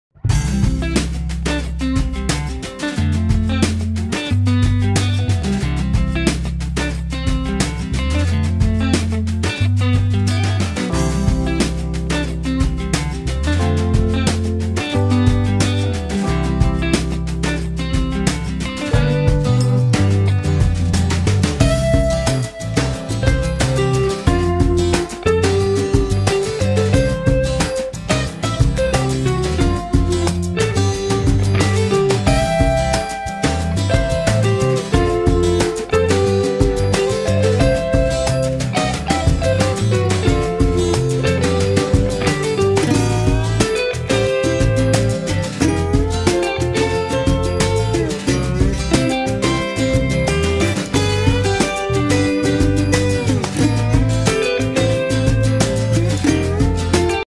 2021 Kayo 150 Storm - New ATV For Sale - Milwaukee, WI
Please visit Southeast Sales Powersports to view this New 2021 Black Kayo 150 Storm for sale in Milwaukee, Wisconsin.

Stock# 003578

Description:
2021 Kayo 150 Storm
The Storm is an awesome for anyone looking for a great value in a solid, quality made ATV. The low-maintenance, air-cooled 140cc engine develops an easy to manage power delivery and it has an electric starter!